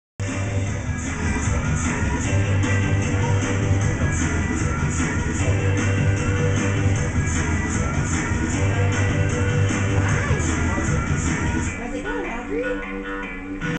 MyGym jumping
Jumping on a trapoline on his 2nd birthday at MyGym.  1/29/09.